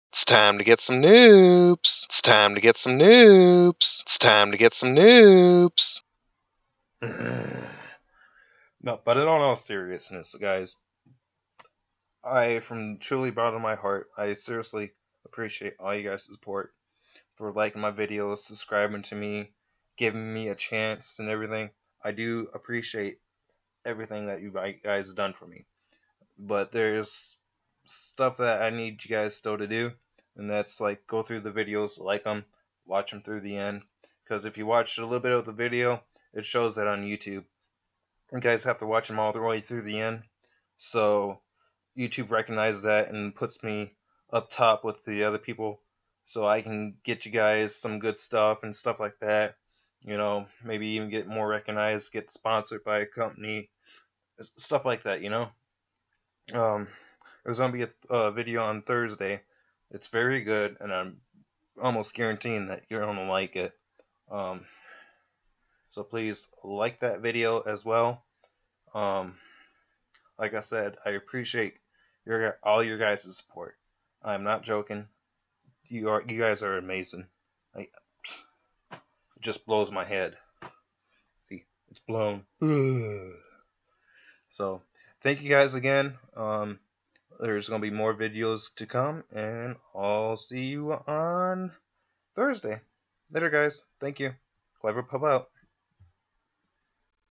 Thank You for 50+ subs! (Clever Pup)
This is just a thank you video for the support and like and subs. I VERY much appreciate it!